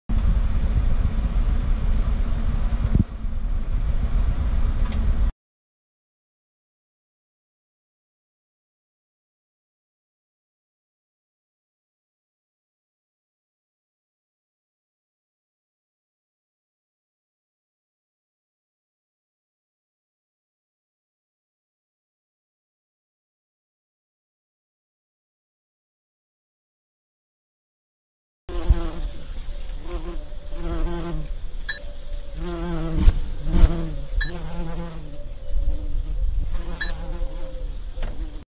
bijenwas wordt hard
Bijna magisch is het om te zien hoe bijenwas hard wordt!